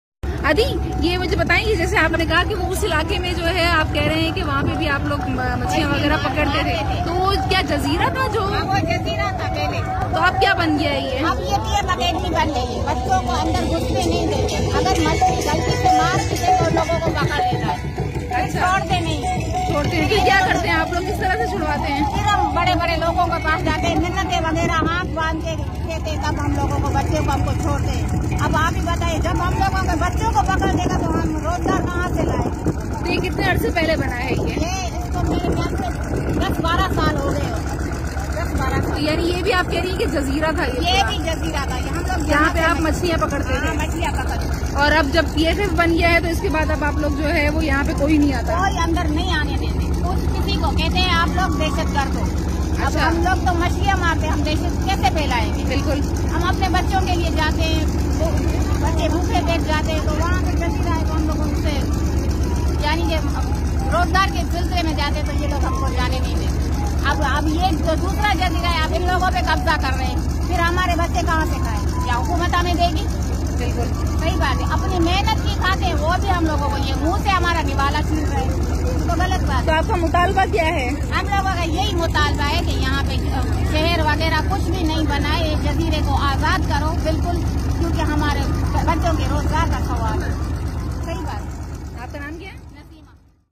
The occupation of Sindh Islands will create an Environmental and Employment problems
سندھ کے جزائر پر قبضہ سے ماحولیاتی اور روزگار کے مسئلے پیدا ہونگے ۔
ایک مختصر رپورٹ جس میں ایک ماہیگیر عورت اپنے دکھ درد بیان کرتے ہوئے۔